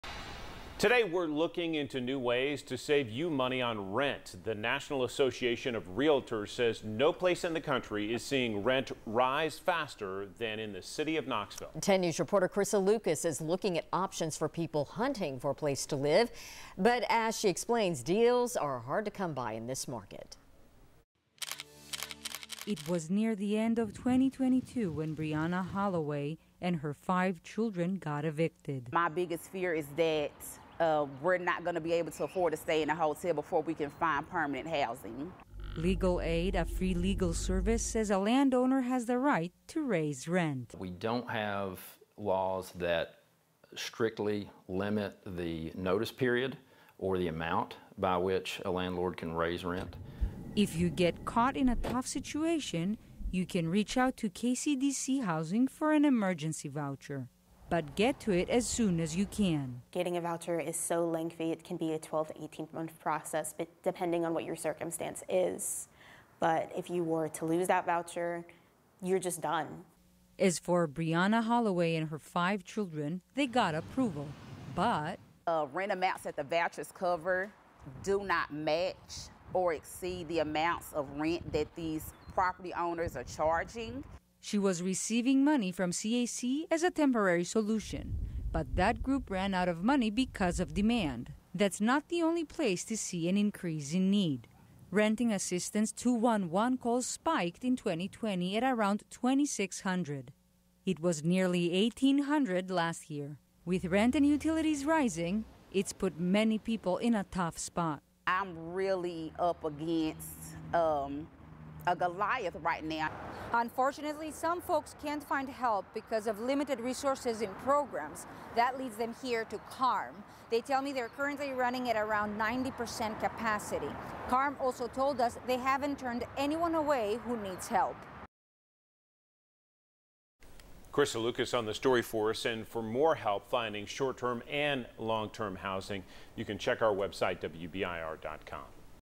Overcoming the 'Goliath' of paying rent while a petition strives to help local renters
When paying rent turns into an uphill battle, finding resources and getting familiar with tenants' rights can help.